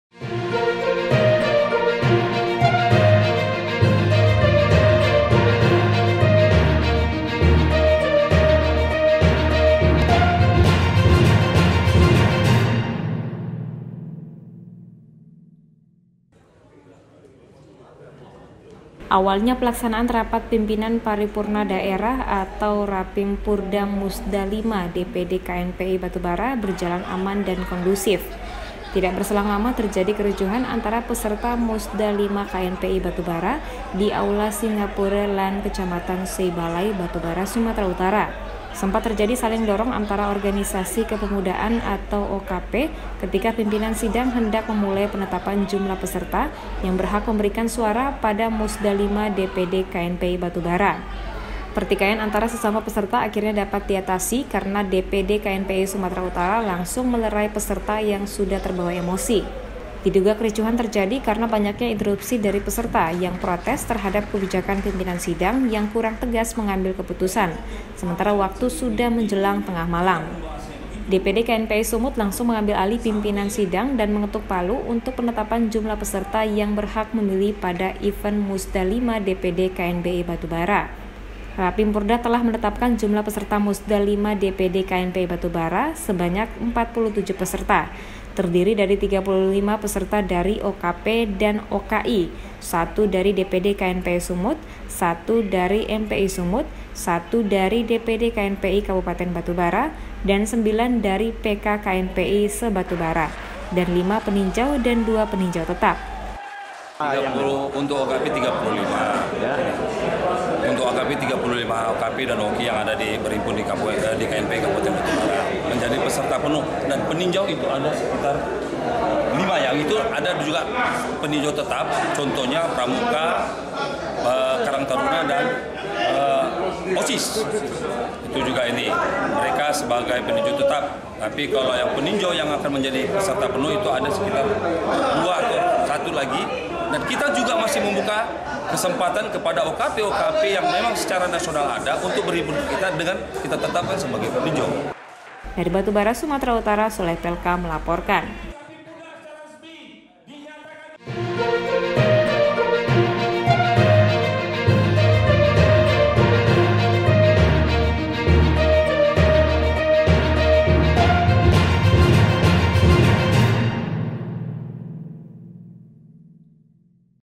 RAPAT PIMPINAN PARIPURNA DAERAH KNPI BB RICUH
Tribrata TV Sumut

Sun, Nov 7, 6:57 AM (1 day ago)

to me, yadi.3btv

AWALNYA PELAKSANAAN RAPAT PIMPINAN PARIPURNA DAERAH ATAU RAPIMPURDA MUSDA V DPD KNPI BATU BARA BERJALAN AMAN DAN KONDUSIF//

TIDAK BERSELANG LAMA TERJADI KERICUHAN ANTARA PESERTA MUSDA V KNPI BATU BARA/DIAULA SINGAPURE LAND/KECAMATAN SEI BALAI/BATU BARA/SUMATERA UTARA//

SEMPAT TERJADI SALING DORONG ANTARA ORGANISASI KEPEMUDAAN ATAU OKP/KETIKA PIMPINAN SIDANG HENDAK MEMULAI PENETAPAN JUMLAH PESERTA YANG BERHAK MEMBERIKAN SUARANYA PADA MUSDA V DPD KNPI BATU BARA//

PERTIKAIAN ANTAR SESAMA PESERTA AKHIRNYA DAPAT DIATASI/KARENA DPD KNPI SUMATERA UTARA/LANGSUNG MELERAI PESERTA YANG SUDAH TERBAWA EMOSI//

DIDUGA KERICUHAN TERJADI KARENA BANYAKNYA INTERUPSI DARI PESERTA/YANG PROTES TERHADAP KEBIJAKAN PIMPINAN SIDANG YANG KURANG TEGAS MENGAMBIL KEPUTUSAN/SEMENTARA WAKTU SUDAH MENJELANG TENGAH MALAM//

DPD KNPI SUMUT LANGSUNG MENGAMBIL ALIH PIMPINAN SIDANG/DAN MENGETUK PALU UNTUK MENETAPKAN JUMLAH PESERTA YANG BERHAK MEMILIH PADA EVEN MUSDA V DPD KNPI BATU BARA//

WAKIL KETUA DPD KNPI SUMATERA UTARA/BUDI NASUTION/MENGATAKAN/DIDALAM MUSYAWARAH PEMUDA HAL YANG BIASA KALAU ADA HUJAN INTERUPSI/NAMUN MASIH DALAM KONTEKS MUSDA/DAN PADA MALAM INI PIMPINAN SIDANG LANGSUNG DIAMBIL ALIH OLEH DPD KNPI SUMUT/DAN MALAM INI RAPIMPURDA SUDAH FINAL/JUMLAH PESERTA DAN PENINJAU SUDAH DITETAPKAN/BESOK MASUK AGENDA MUSDA//

RAPIMPURDA TELAH MENETAPKAN JUMLAH PESERTA MUSDA V DPD KNPI BATU BARA 47 PESERTA//
TERDIRI DARI 35 PESERTA DARI OKP DAN OKI/SATU DARI DPD KNPI SUMUT/SATU DARI MPI SUMUT/SATU DARI DPD KNPI KABUPATEN BATU BARA DAN 9 DARI PK KNPI SE BATU BARA/ ADA 5 PENINJAU DAN DUA PENINJAU TETAP//
INFORMASI DARI PANITIA BAKAL CALON KETUA KNPI BATU BARA YANG TELAH MENDAFTAR ADA TIGA ORANG//
MUKHRIJAL ARIF/OKY IQBAL FRIMA/DAN FADLI MAHSAN//